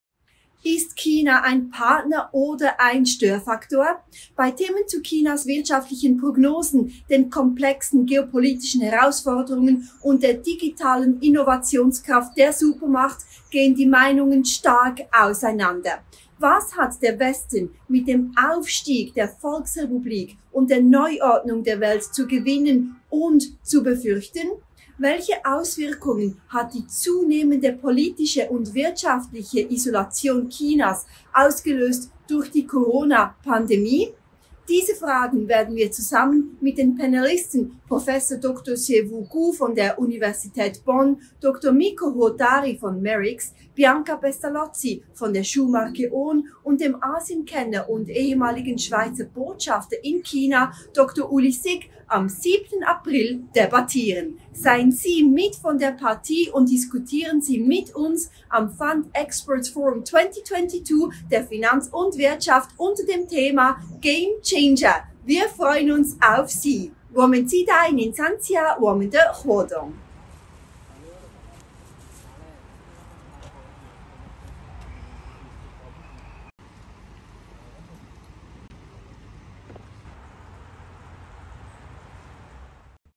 Martina Fuchs - Preview zum Fund Experts Forum 2022
Referenten des Forums:
-Marc Elsberg Bestseller author (incl. «Blackout»)
-Prof. Dr. Xuewu Gu Director International Relations/Center for Global Studies (CGS) Bonn University
-François Hollande Président de la République Française/President of France (2012–2017)
-Dr. Mikko Huotari Director MERICS
-Prof. Dr. Dr. h.c. Uwe Krueger Head EMEA Temasek International Pte.
-Bianca Pestalozzi General Manager Asia-Pacific ON
-Dr. Uli Sigg former Swiss ambassador to China, North Korea and Mongolia and eminent Asia expert

Moderation:
Martina Fuchs TV Anchor & Business Journalist (Xinhua)
Jan Schwalbe Editor-in-chief Finanz und Wirtschaft